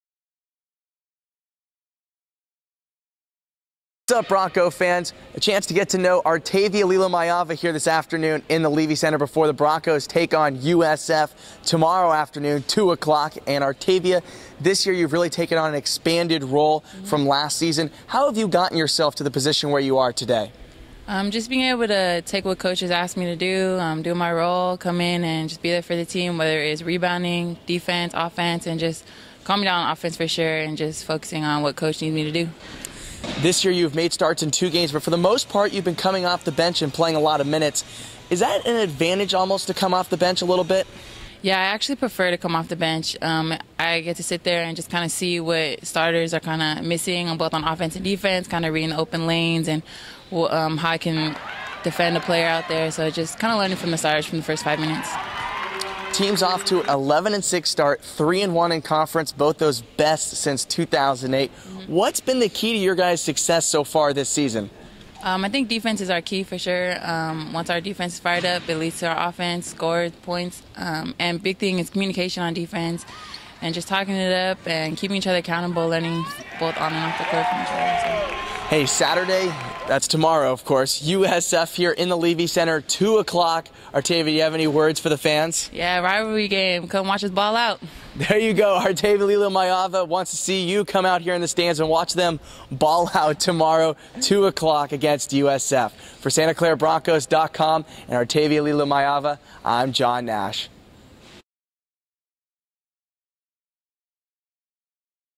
Chatting with Arteivia Lilomaiava
The Voice of Santa Clara women's basketball John Nash chats with Arteivia Lilomaiava about her new role this season as well as the team's 11-6 start to the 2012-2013 season.